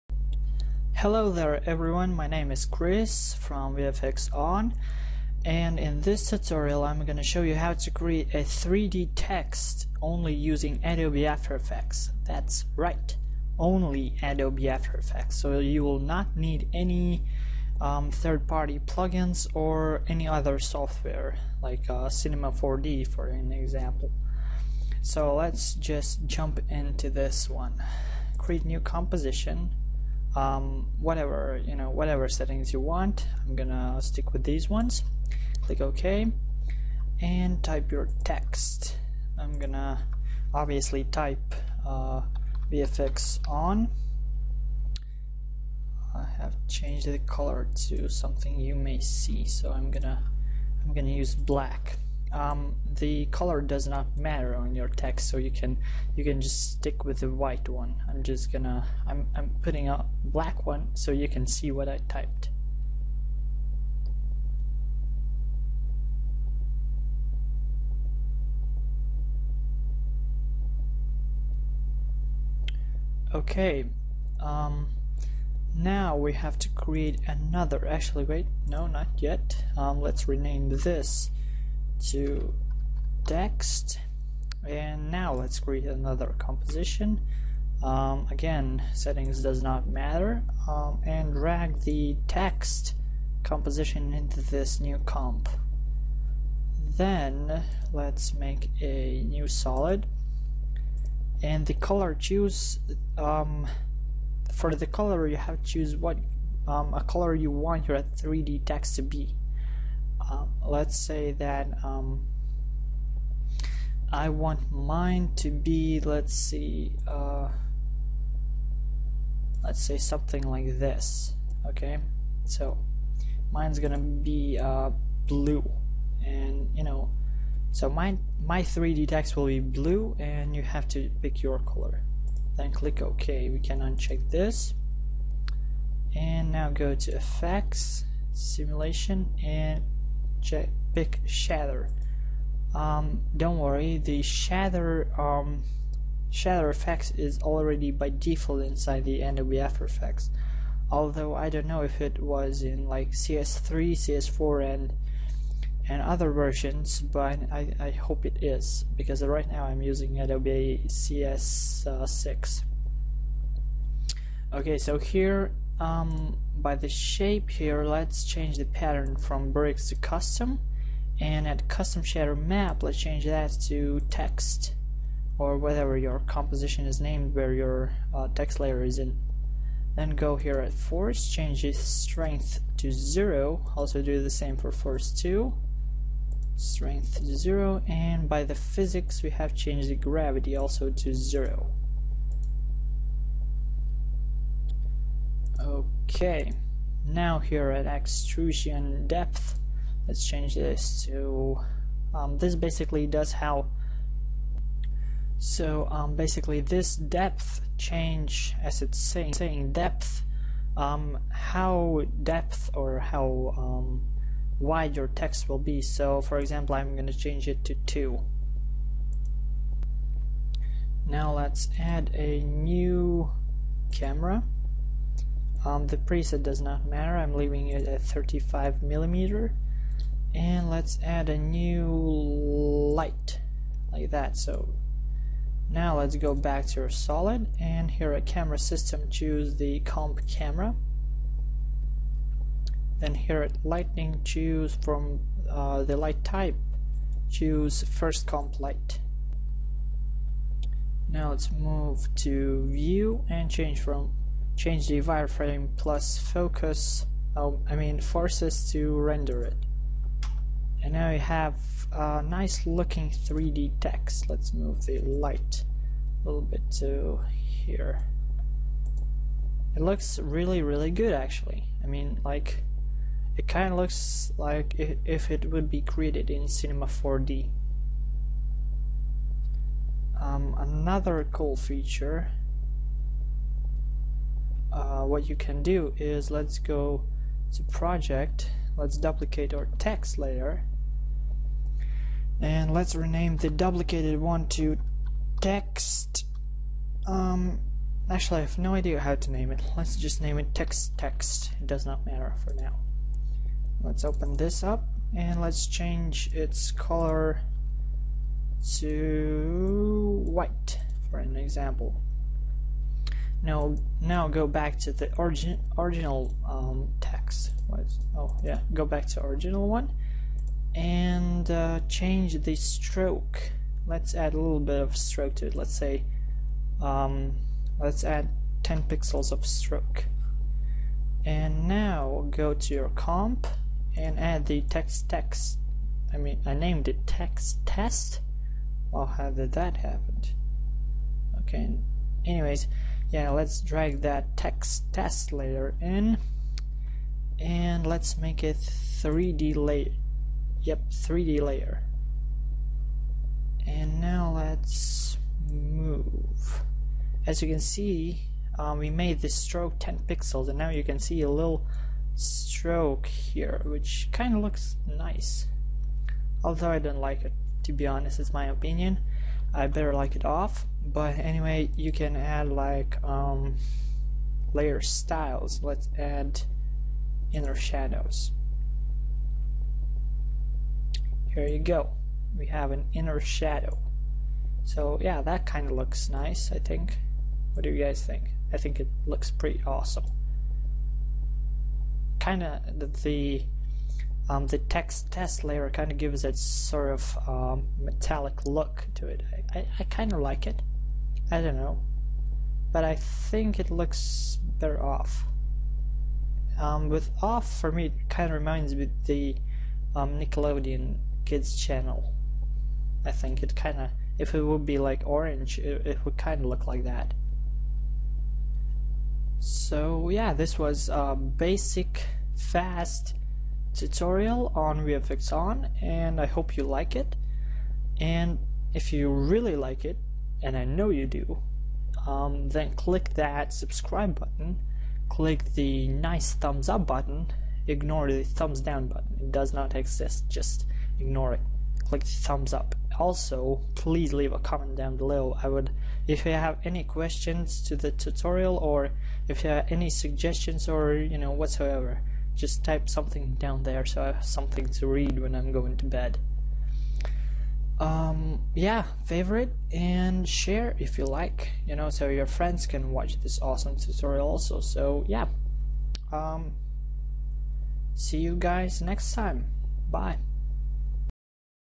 After Effects 3D Text Tutorial
3D Text tutorial. Make 3D Text using only After Effects.
No need for Cinema 4D or 3DS Max.